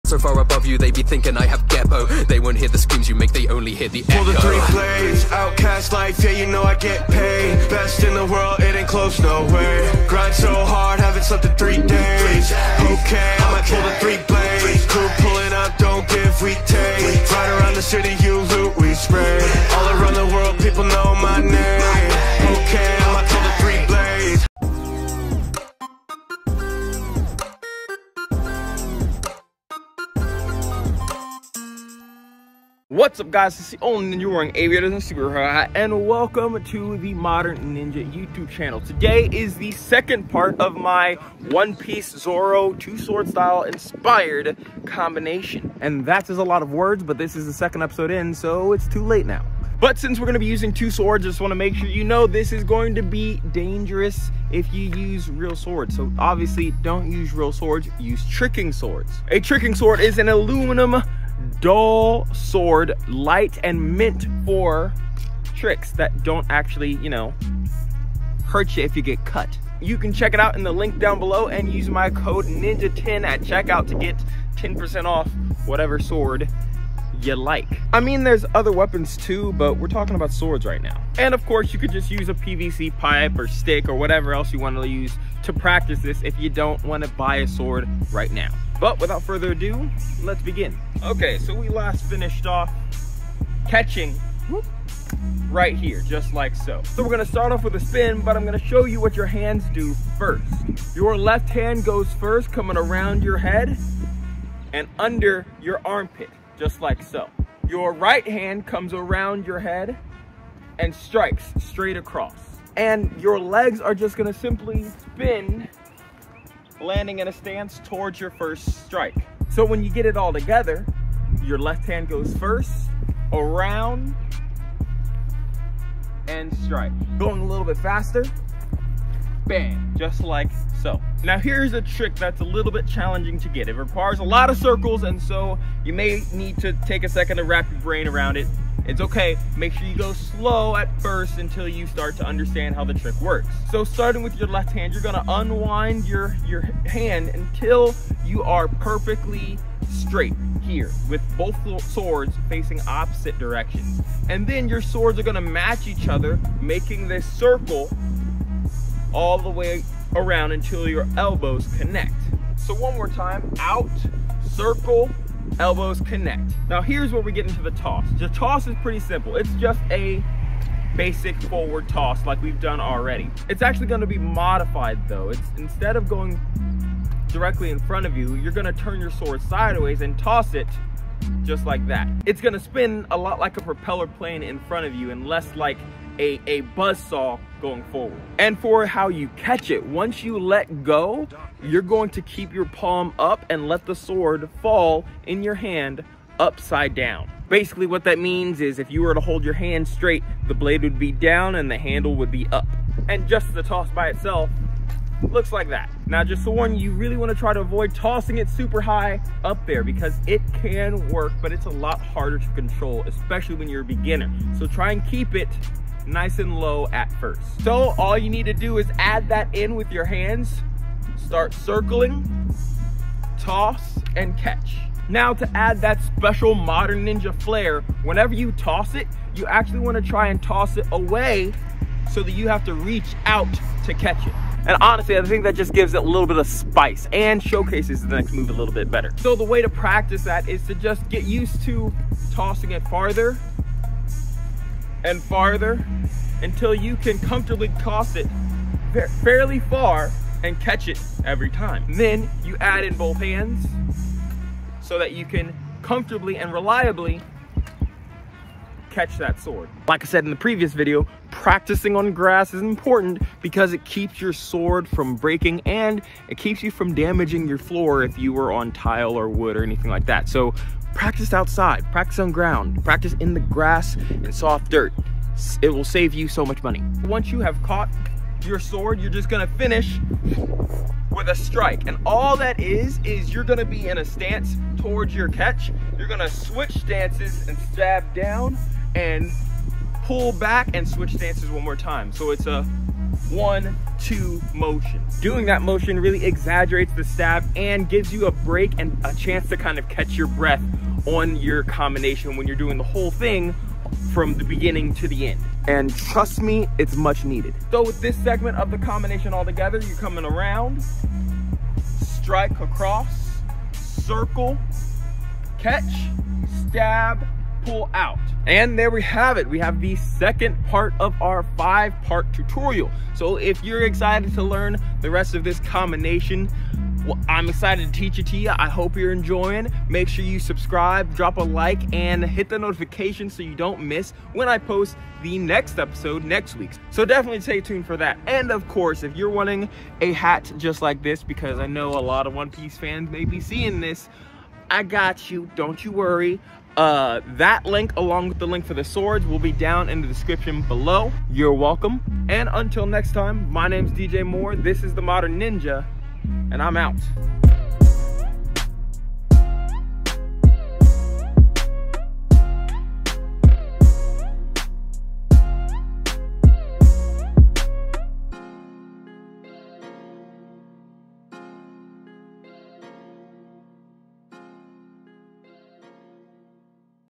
Zoro's Two Sword Style Combination (2of5) | Martial Arts Tutorial
This month we are going to be breaking down a dual sword combination inspired by one of my favorite anime characters. Roronoa Zoro from The Straw Hat Pirates on One Piece. So if you want to learn an awesome katana trick then get your xma stunt swords ready and lets do it!